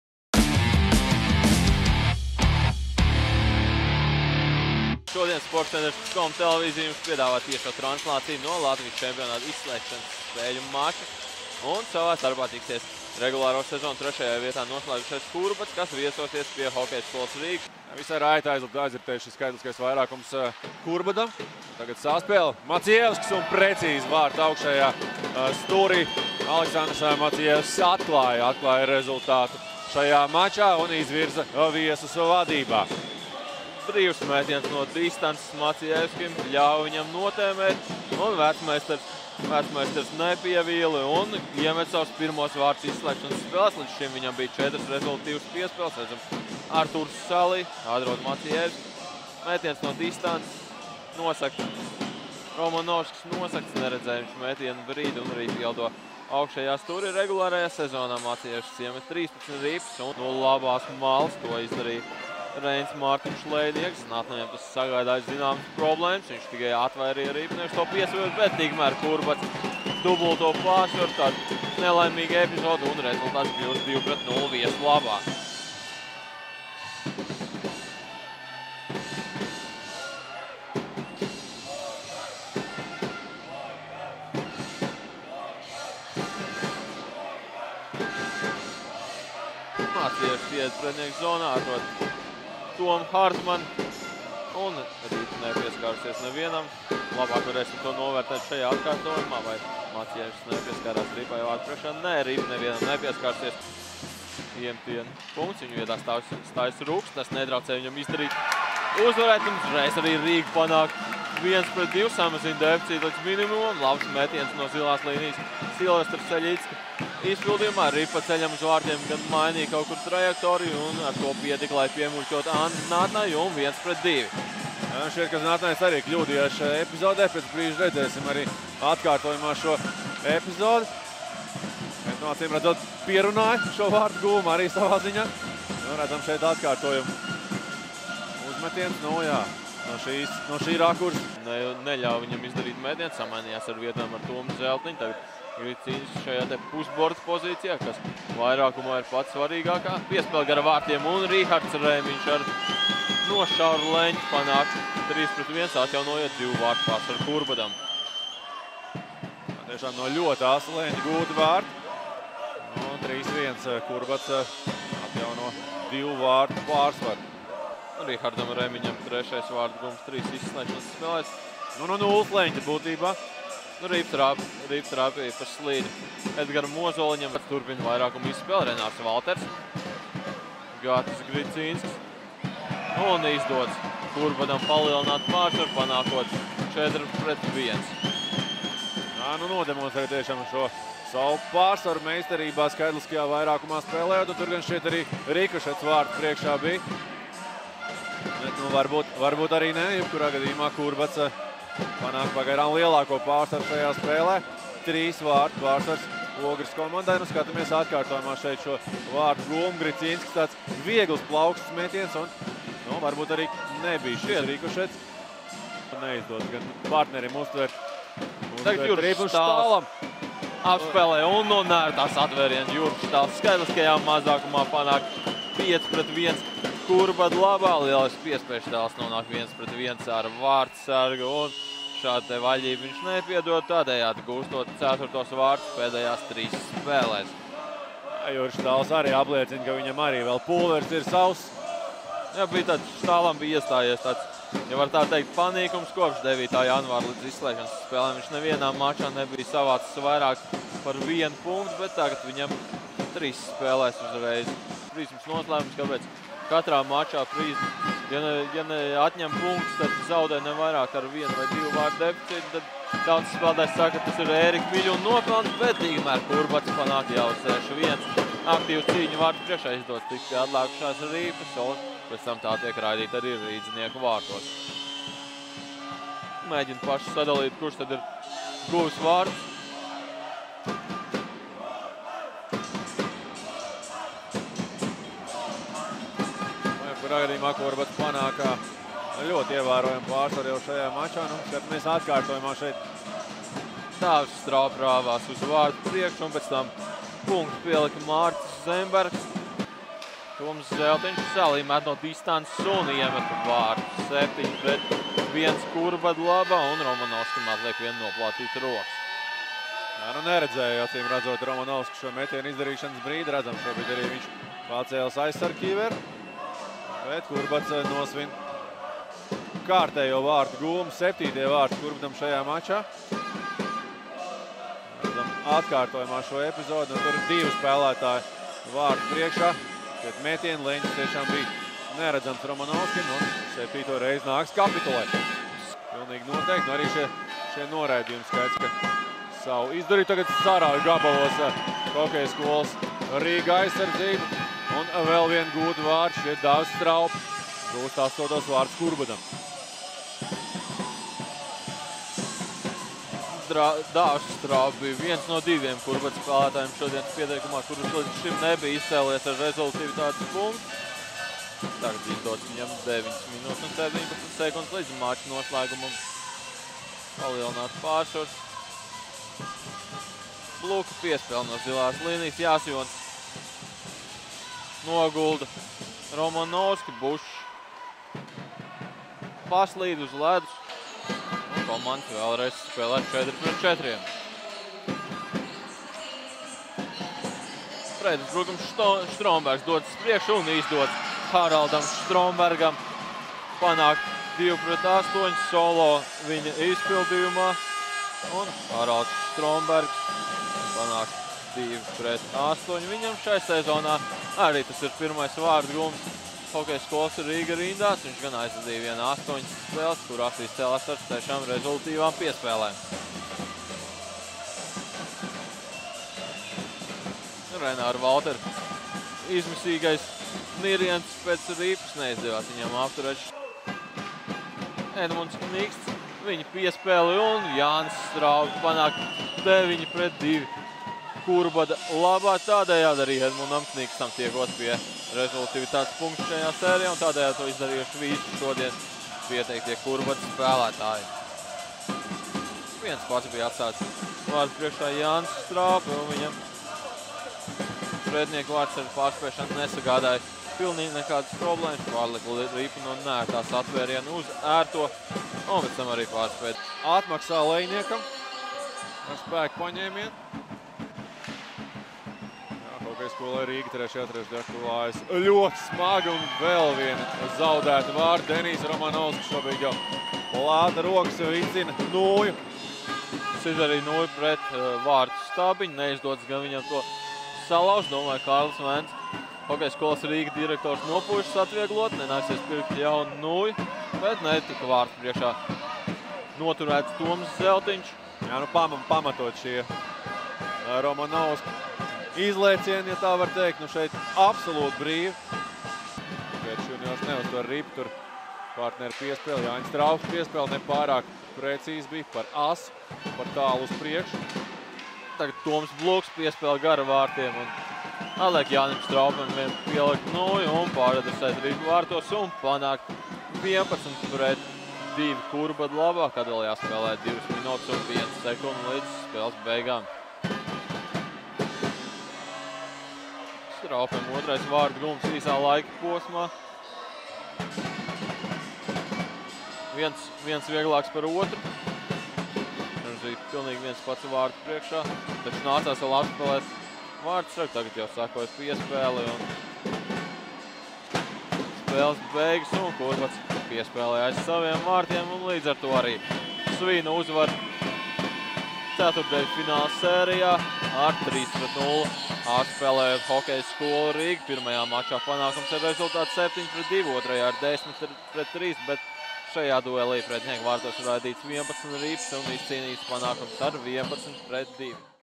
Kurbads vs HS Rīga (11-2 hockey highlights) 12.03.2016
Sestdienas pēcpusdienā hokeja hallē „Rīga” trešo un kā izrādījās noslēdzošo ceturtdaļfināla spēli aizvadījas hokeja klubs „Kurbads” un hokeja skola „Rīga”. Lai arī divas trešdaļas rezultāts turējās gana līdzīgs, spēles noslēdzās ar pārliecinošu viesu uzvaru ar 11:2.

Ar četriem rezultativitātes punktiem ražīgākais HK „Kurbads” spēlētājs izrādījās Toms Hartmanis, bet ar trim punktiem atzīmējās Toms Bluks un Gatis Gricinskis.

Par viesu izteikto pārsvaru liecina gan metienu attiecība (22:51), gan spēka paņēmieni (10:19).

HK „Kurbads” iekļūst pusfinālā, kur pirmā cīņa ar Mogo norisināsies 18. martā Inbox ledus hallē.